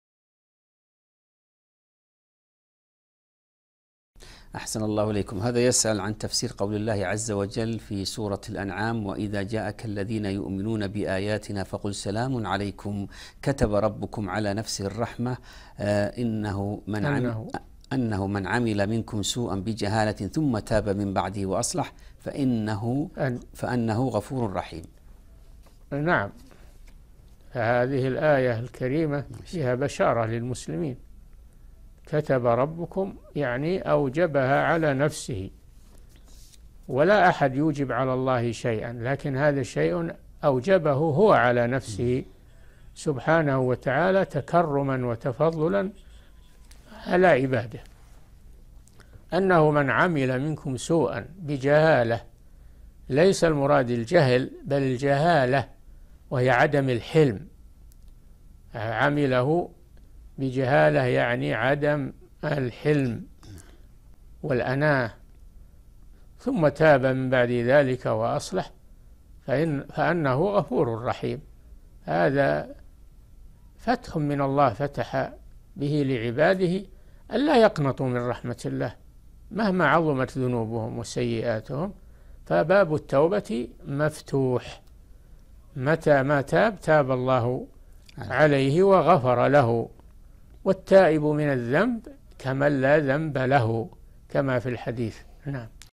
تفسير قوله تعالى (وإذا جاءك الذين يؤمنون بآياتنا فقل سلام عليكم..) الآية - الشيخ صالح الفوزان
-نص السؤال: هذا يسأل عن تفسير قول الله عز وجل في سورة الأنعام )وإذا جاءك الذين يؤمنون بآياتنا فقل سلامٌ عليكم كتب ربكم على نفسه الرحمة أنه من عمل منكم سوءاً بجهالةٍ ثم تاب من بعده وأصلح فأنه غفور رحيم)
السيرة الذاتية :
للاشتراك في قناة الشيخ صالح الفوزان
لمشاهدة حلقات فتاوى على الهواء كاملة

للاستفسار والإقتراحات والدعم
رئيس الفريق
لمشروع كبار العلماء